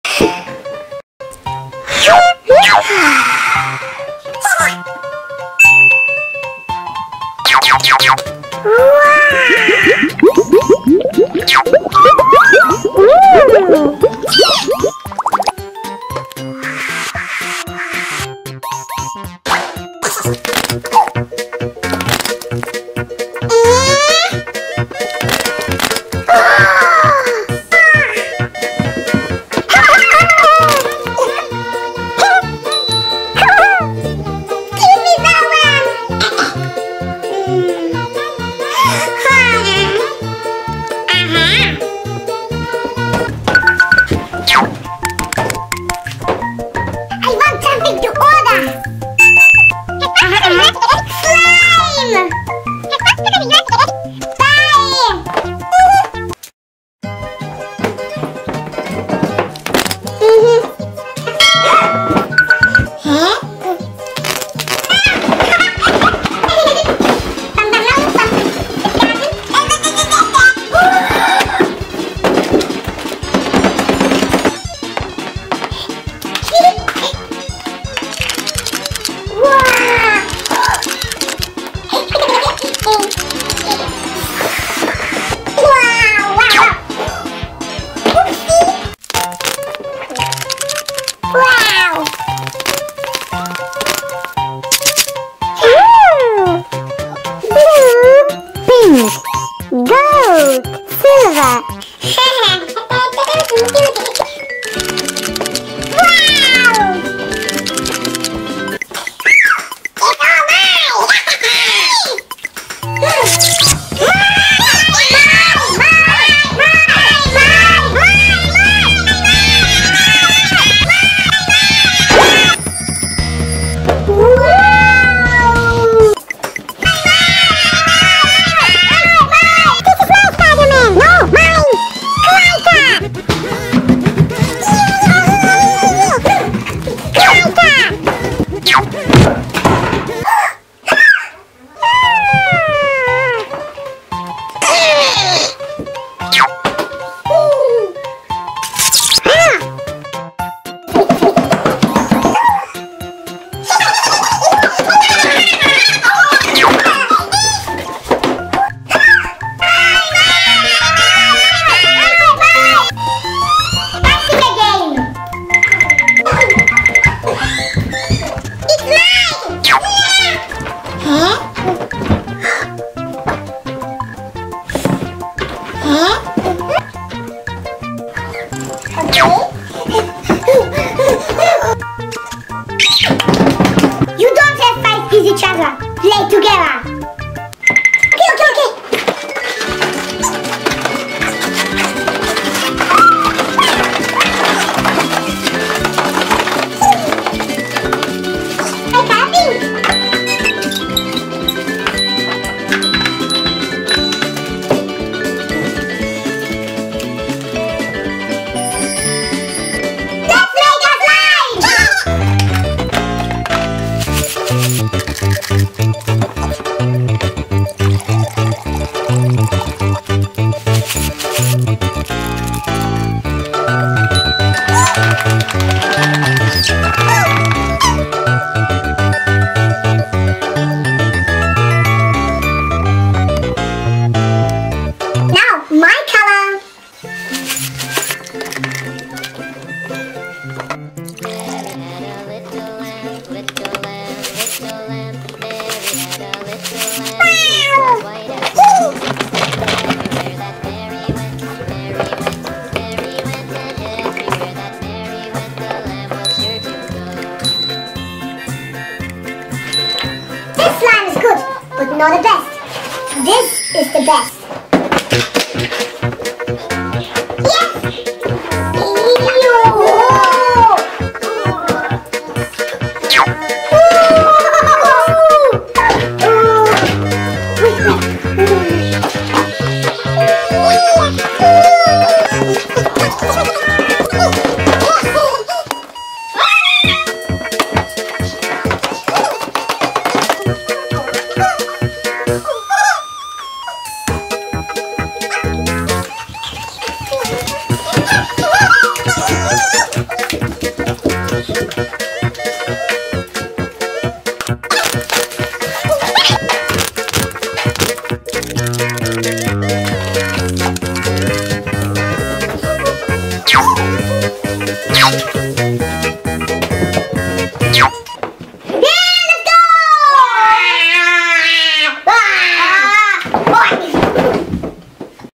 Nastya | Funny Story with Slimes for kids Nastya makes colored slime Pretend play with giant slime
Nastya and Alex make colored slime and pretend play with giant slime Funny Story with Slimes for kids  Настя делает цветной слайм
Learn colors with Nastya

Nastya - fun adventure with a slime